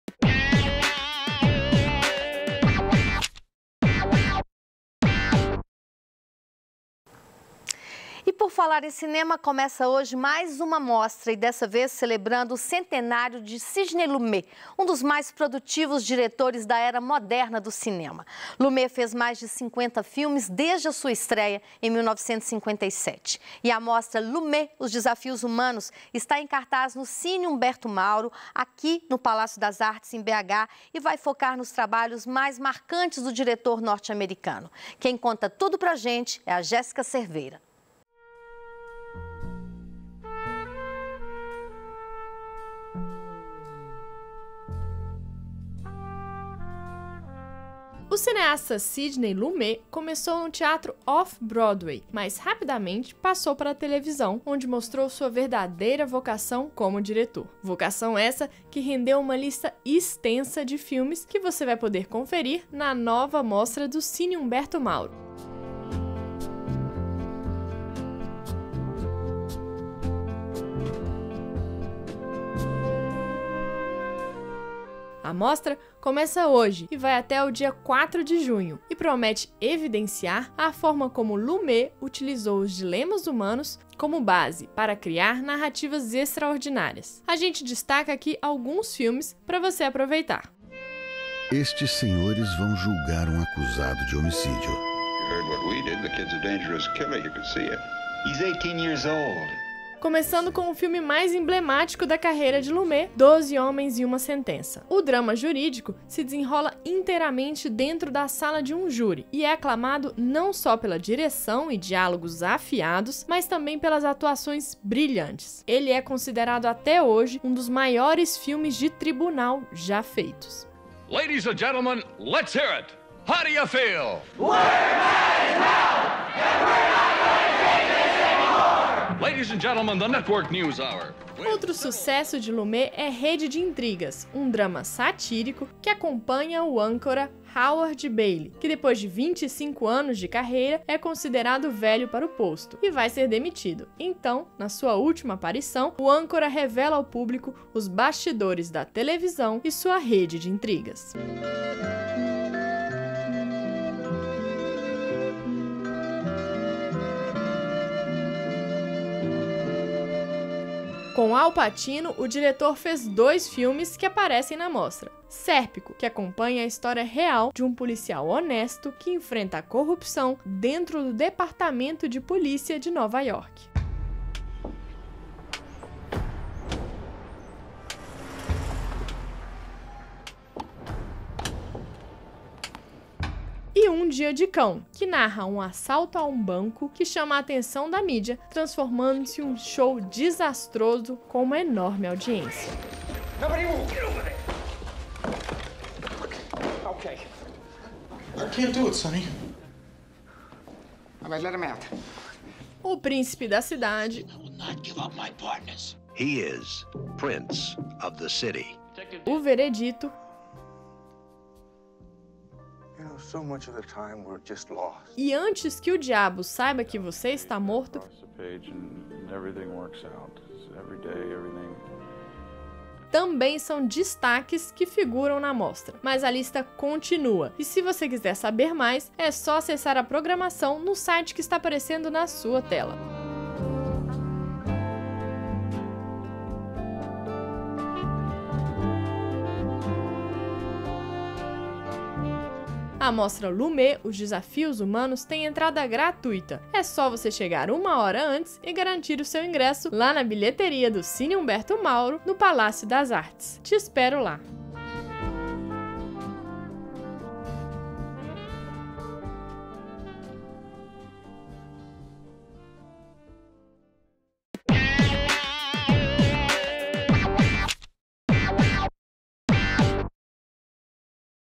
MOSTRA Centenário Sidney Lumet
Exibido em 14 de Maio
Está começando hoje mais uma mostra de cinema, dessa vez celebrando o centenário de Sidney Lumet, um dos mais produtivos diretores da era moderna do cinema.
Lumet fez mais de 50 filmes desde sua estreia em 1957, e a mostra "Lumet: os desafios humanos", está em cartaz no Cine Humberto Mauro e vai focar nos trabalhos mais marcantes do diretor norte americano. Quem conta tudo pra gente é Jéssica Cerveira.
Confere aí!